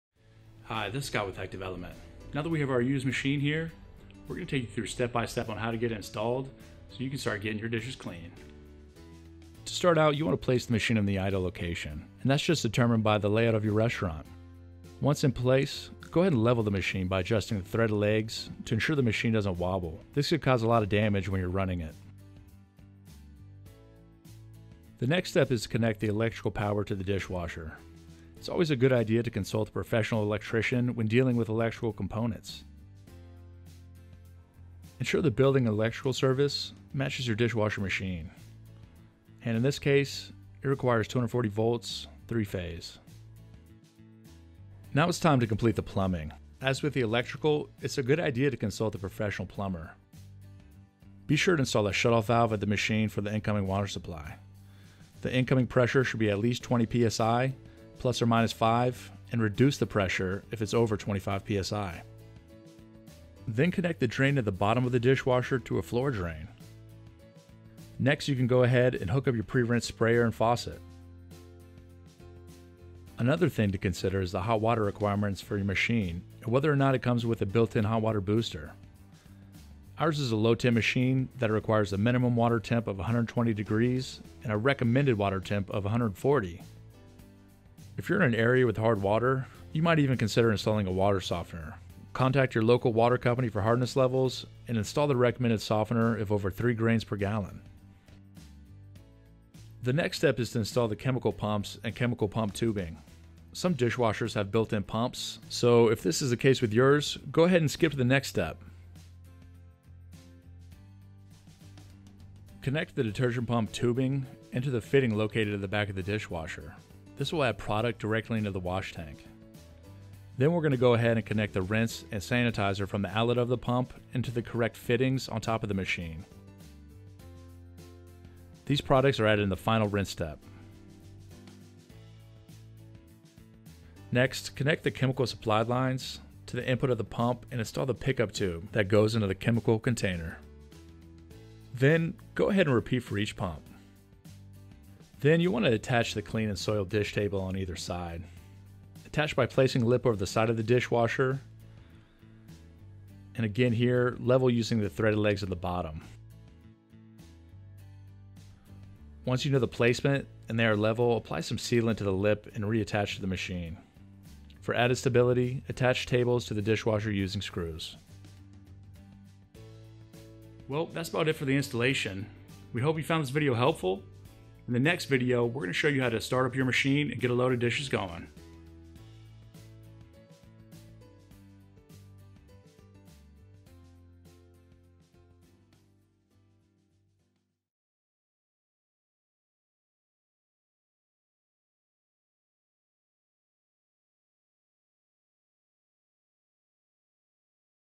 How to Install a Commercial Dishwasher - Active Element - Maintenance Series
In this video, we will guide you step by step on how to install a commercial dishwasher. We actually bought a used machine to just to make this video!

Installing your own machine is easy and can save you tons of money than leasing. After watching this you can start up your machine and start cleaning dishes in your commercial kitchen. Several considerations including electric, plumbing, dish tables, chemical pumps and more. Great for any commercial kitchen including restaurants, diners, bars, not-for-profits and schools.

Hope you enjoy the video and Concentrate On Savings!